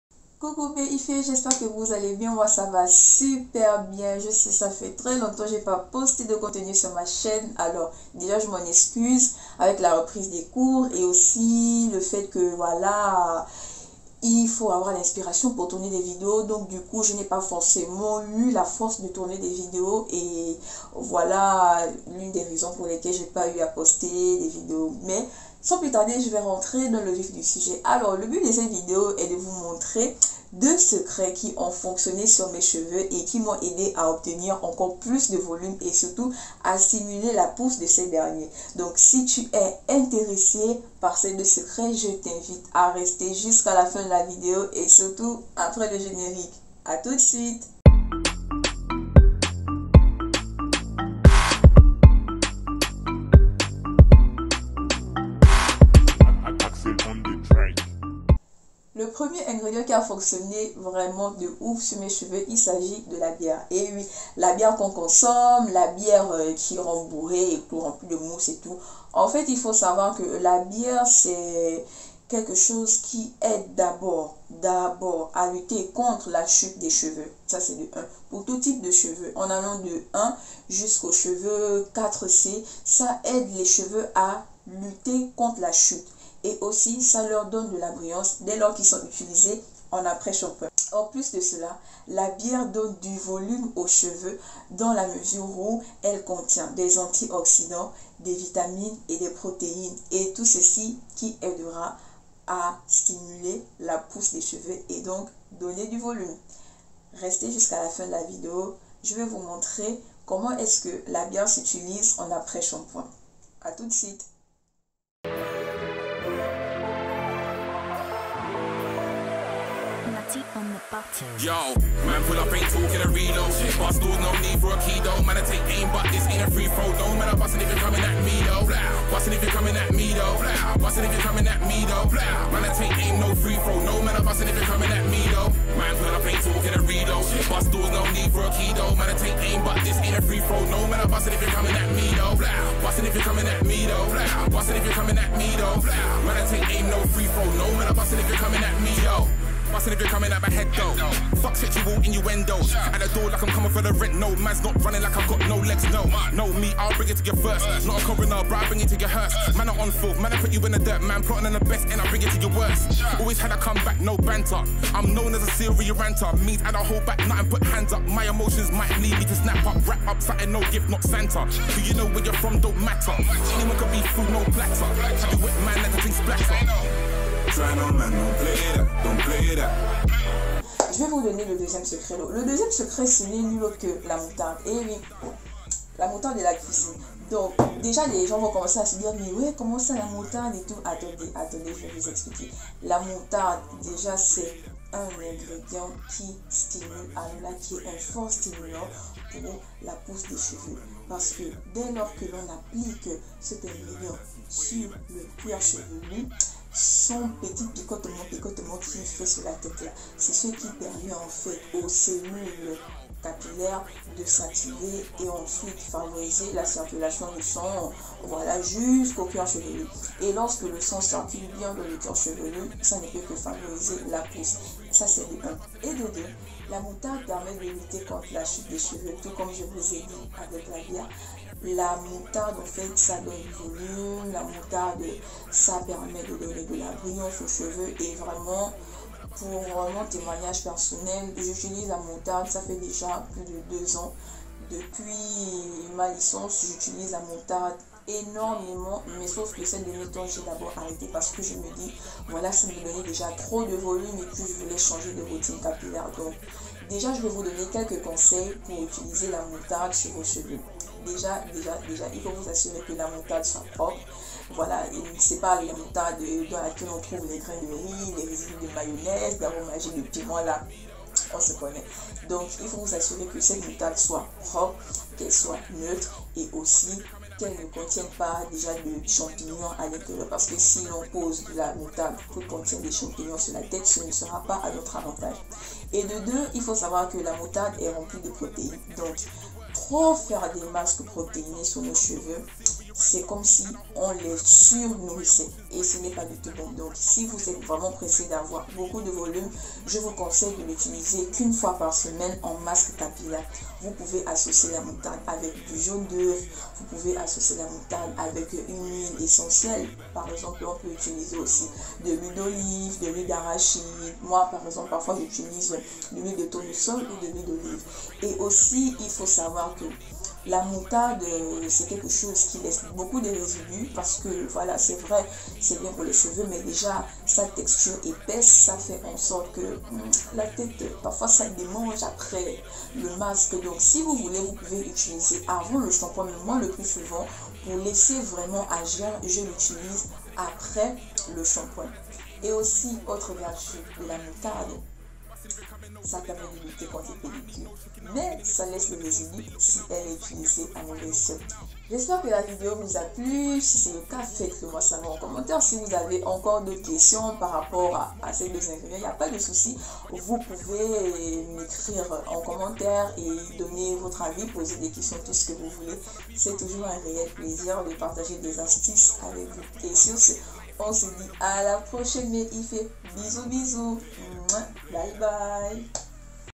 VOICI MES SECRETS QUI DONNERONT A TES CHEVEUX UN VOLUME ET UNE POUSSE DE OUF !!!!!
Booooonjouuuuuurrrr mes Ife j'espère que ça va bien! Voici une vidéo dans laquelle je vous présente des aliments m' ayant permis à avoir une pousse et une pousse incroyables !